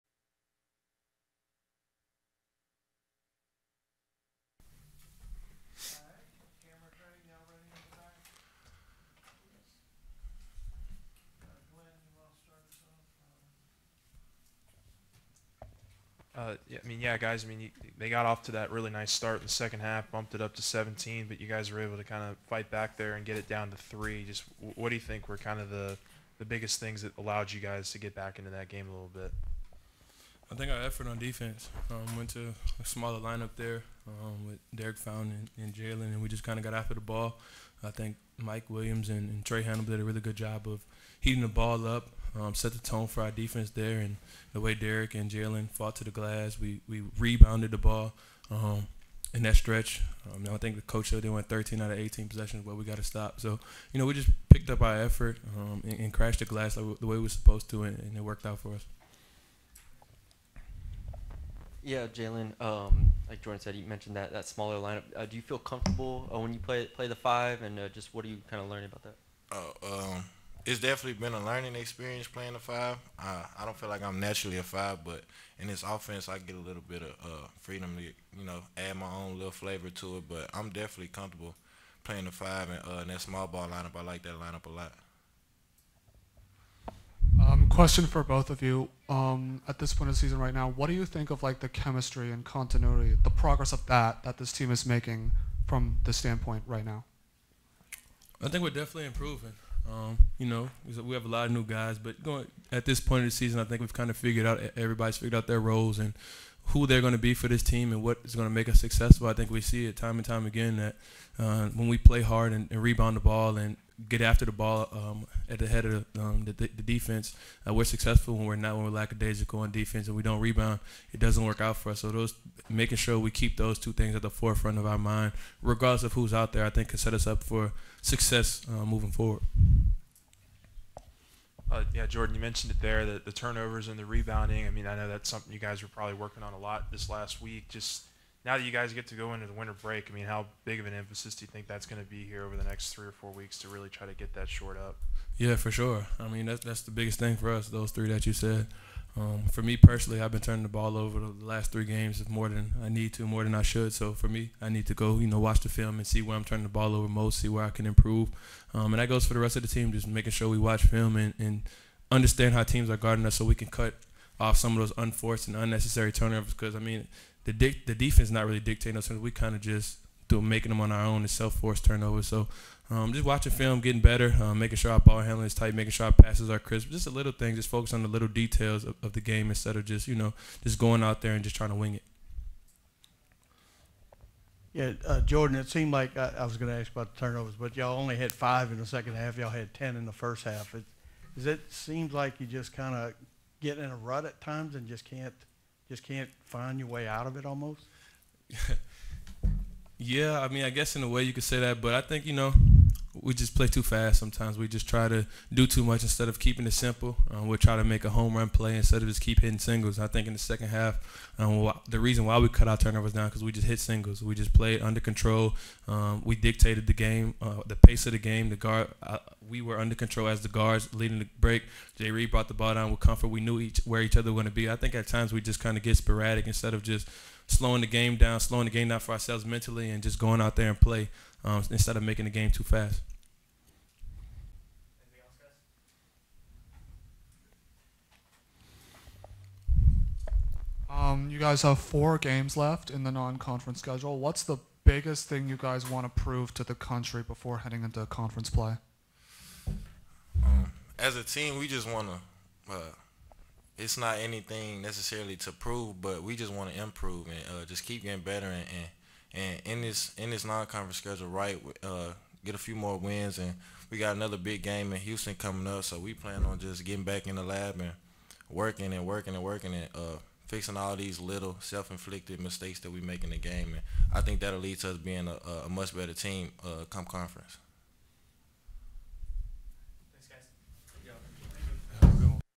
LSU's Jordan Wright and Jalen Reed LOSS to Kansas State postgame
LSU men's basketball players Jordan Wright and Jalen Reed spoke with the media following the Tigers 75-60 loss to Kansas State.